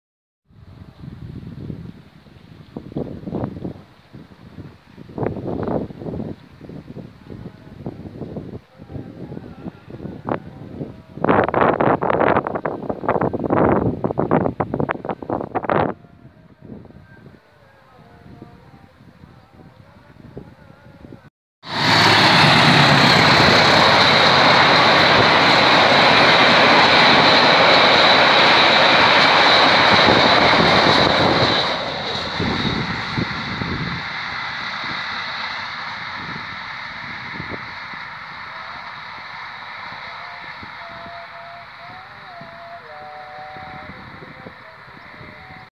WUSS... ABA LEWAT
Hunting siang kali saya berada di Barat Stasiun Ngrombo melintas KA. Argo Bromo Anggrek dari Surabaya ke Jakarta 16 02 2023.